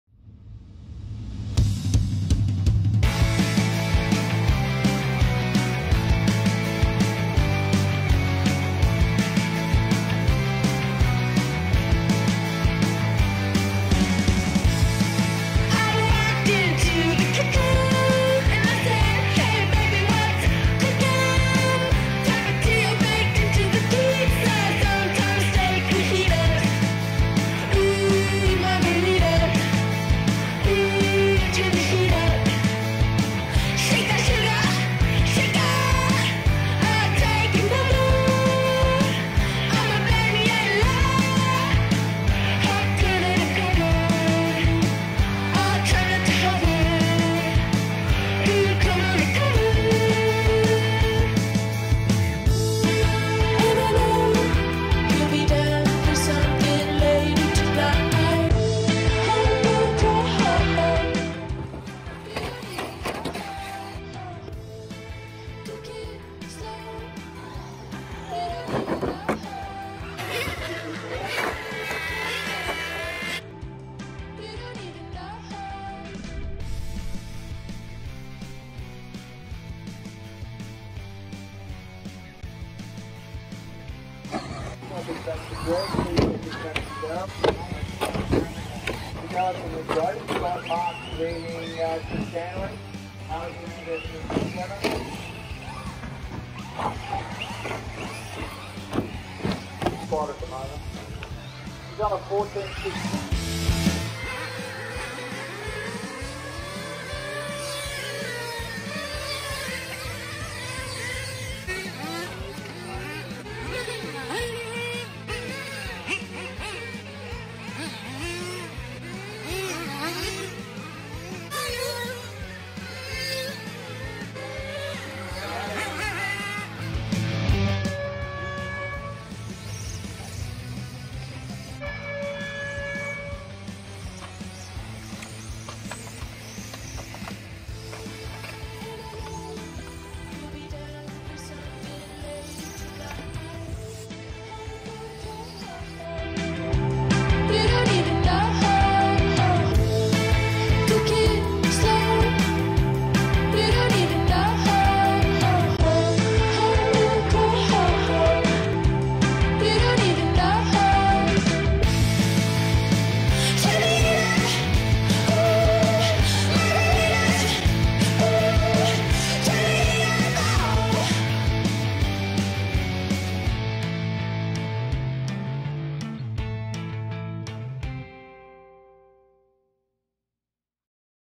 Highlights - Day 1 (Saturday) - Smokem 2021 IMCC Summer Championships (IMCC)
Here is our little highlights video from Day 1 of the Smokem 2021 IMCC Summer Championships hosted superbly by IMCC.

Featured classes were 1/8 GT, 1/10 Nitro Tourer, 1/12th Scale, and Short Course Truck!